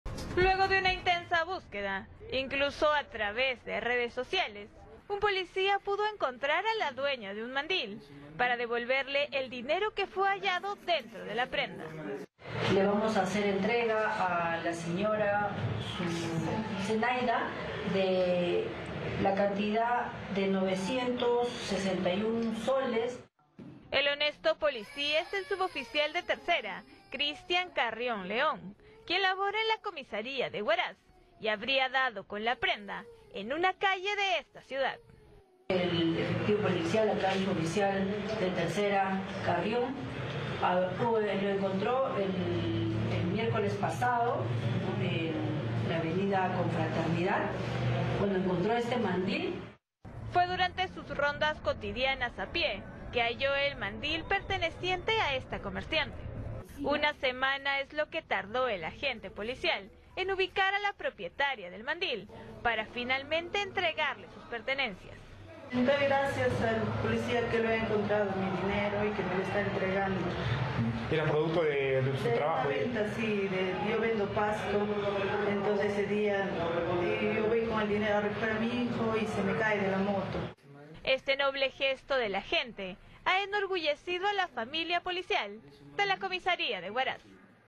¡Gesto ejemplar! Policía devolvió 900 soles a comerciante en Huaraz
¡Gran gesto de honestidad! Luego de buscarla por más de una semana, un policía en Áncash devolvió un mandil con más de 900 soles a comerciante.